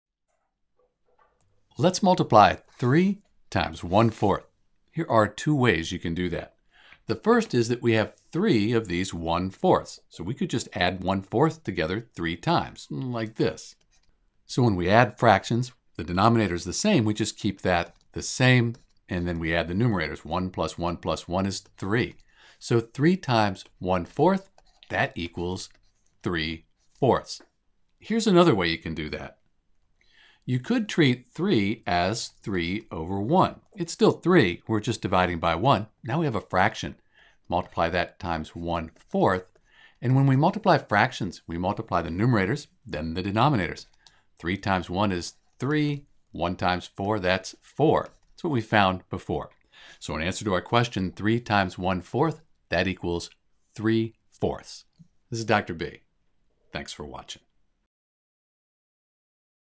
3 times 1/4 (Three times One-Fourth)
When we multiple a number by a fraction we first multiply the numerators and then multiple the denominators.  In this sense multiplying a fraction like 3  x 1/4 is fairly easy.

The trick is to convert 3 to 3/1 so we have a fraction.  Since we are dividing by "1" we haven't change the value, just the way it is written. We then have:
3/1  x   1/4

We multiply the numerators:  3  x 1 = 3
and then the denominators:   1 x 4 =  4

We end up with 3/4  which we cannot simplify further.